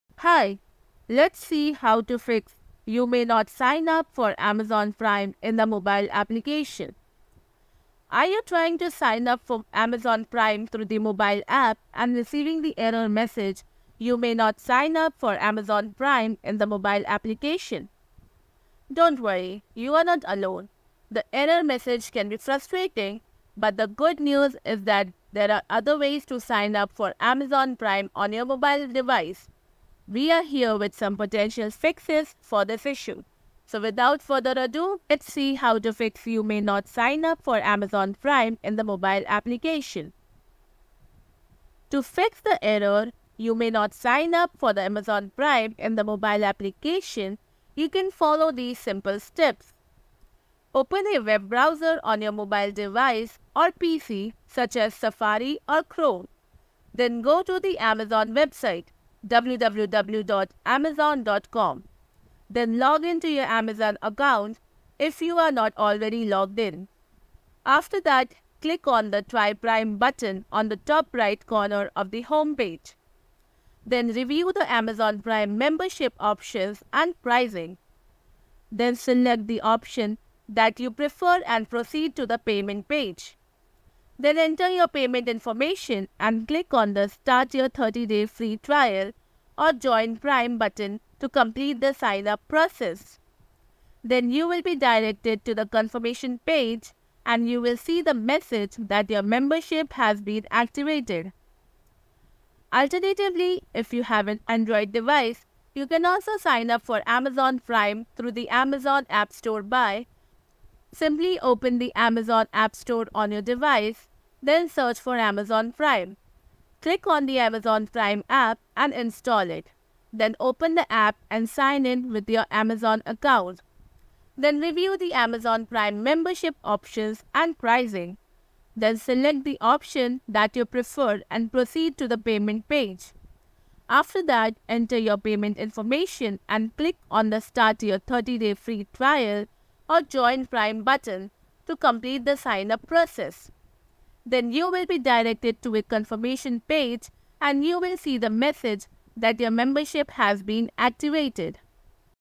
How To Fix You May Not Sign Up For Amazon Prime In The Mobile Application
Trying to sign up for Amazon Prime through the mobile app and encountering the "You may not sign up for Amazon Prime in the mobile application" error? You're not alone! In this video, we'll explain why this issue occurs and provide step-by-step solutions to help you complete your Amazon Prime sign-up on your mobile device with ease.